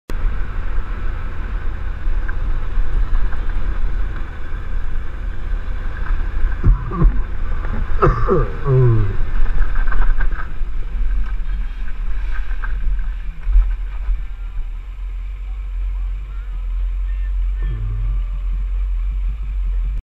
Tonys Slip
Aussie Riders group on Facebook... A few of us went for a ride and a small mishap on some gravel roads coming down to Kennilworth.

Luckily was only cosmetic damage (and ego) but continued to ride on.